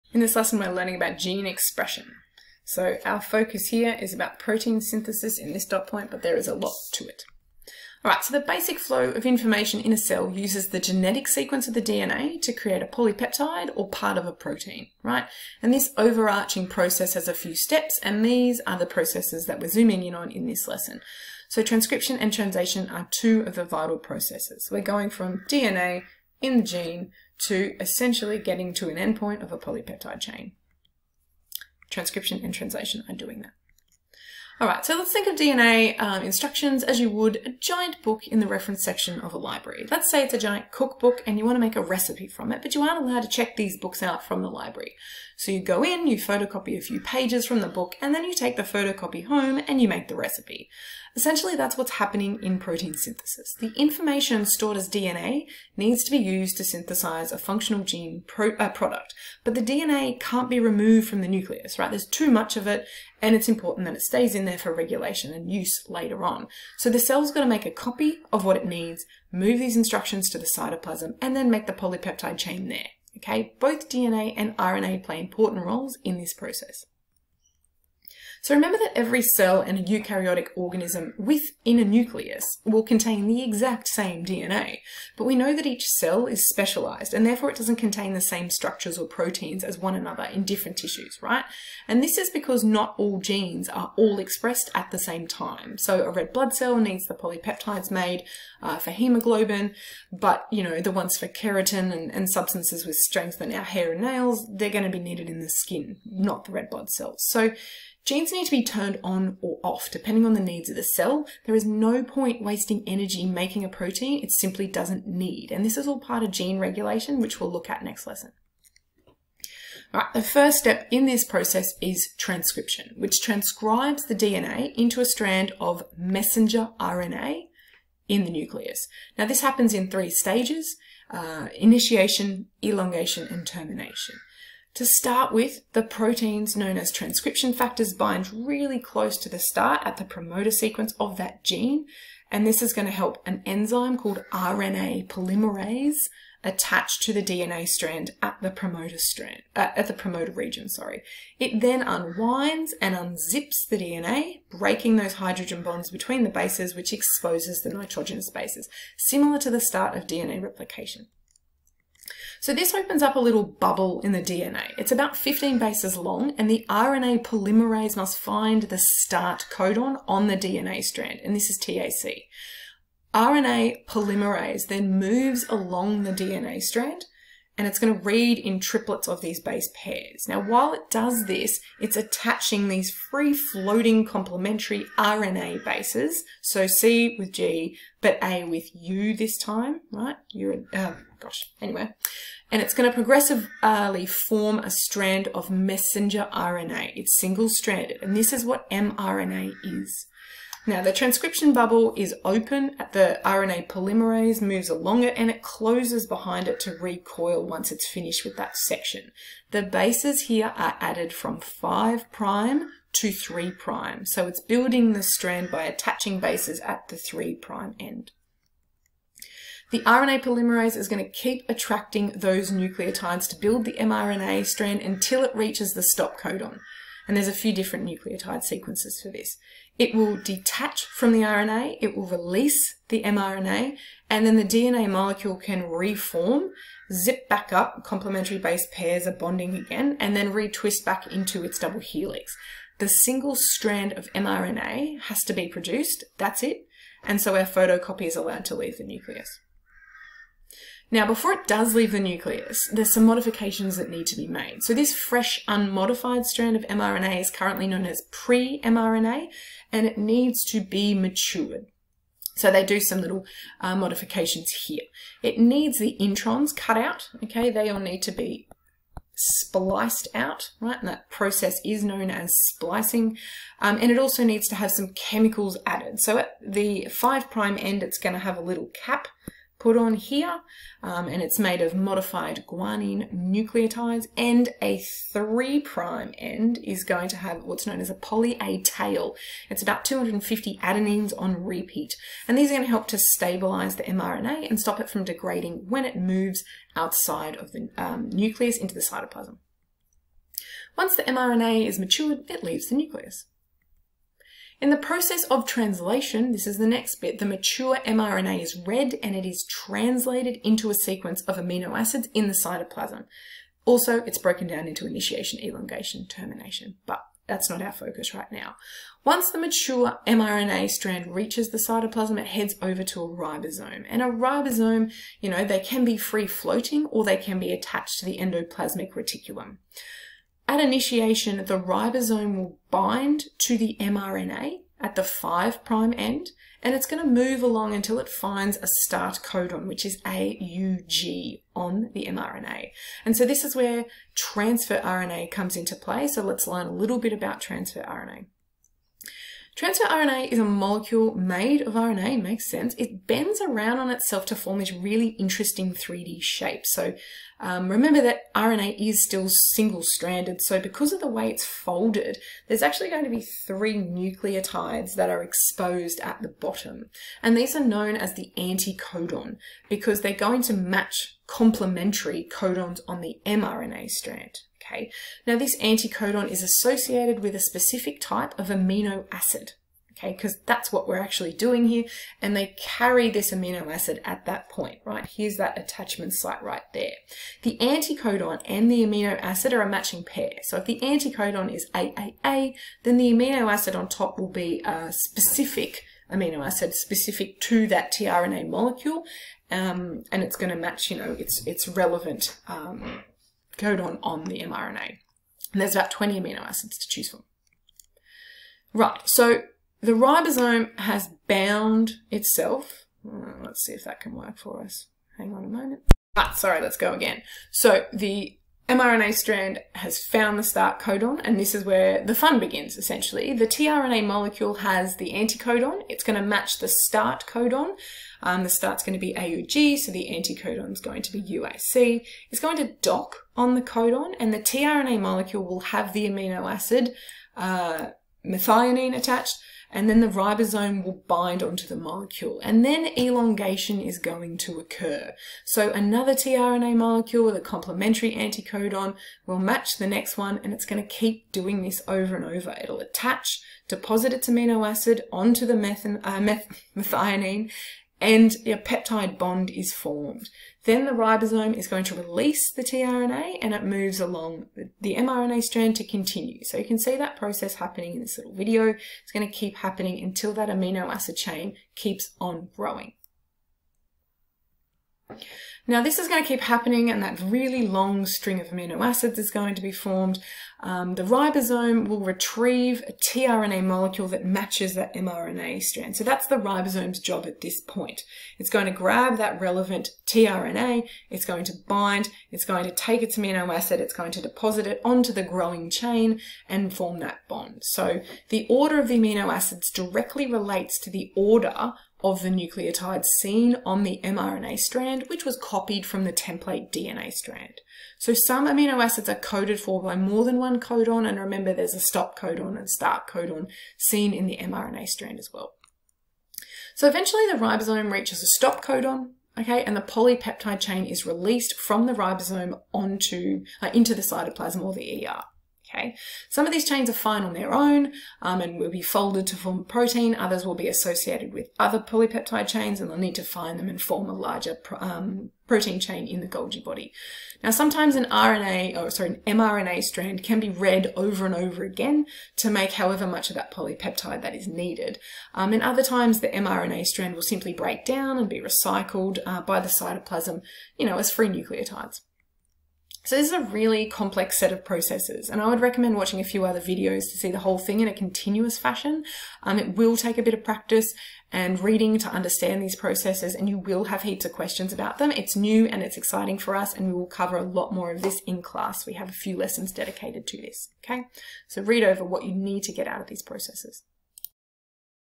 Gene expression
QCAA Biology 2019, Unit 4, Topic 1
- explain the process of protein synthesis in terms of
transcription of a gene into messenger RNA in the nucleus
translation of mRNA into an amino acid sequence at the ribosome (refer to transfer RNA, codons and anticodons)